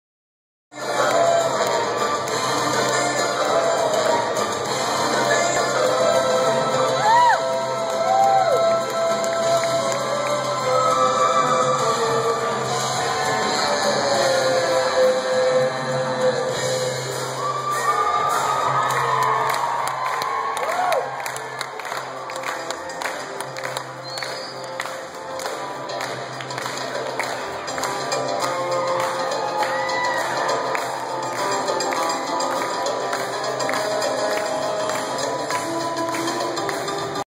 Ozomatli playing with Colorado Symphony Orchestra
Clip from Ozomatli playing with the Colorado Symphony on Nov 12, 2011 in Denver